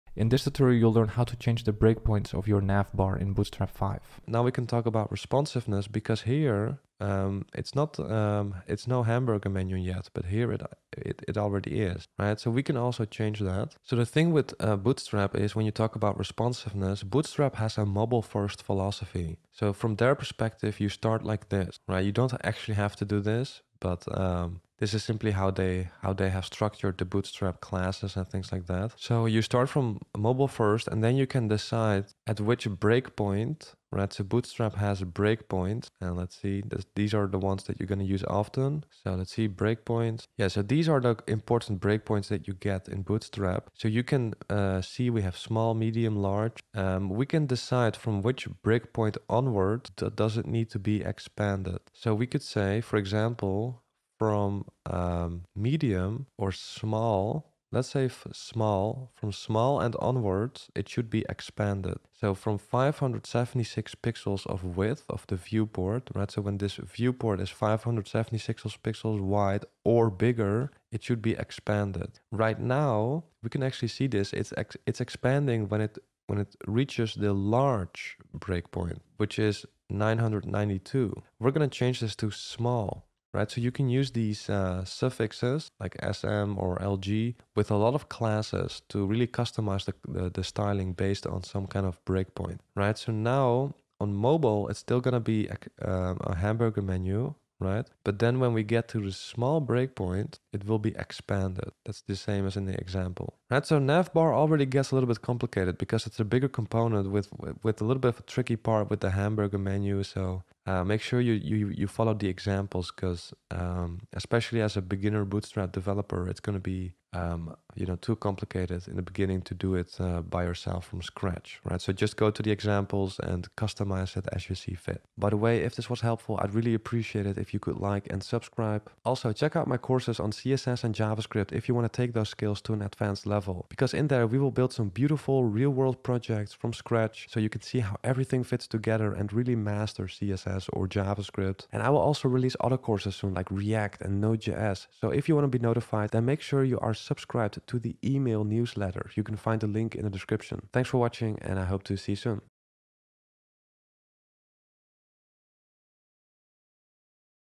Bootstrap 5 Navbar - Change Breakpoint
In this tutorial you’ll learn how to change the responsiveness breakpoint of the navbar component in Bootstrap 5.

0:00 Explanation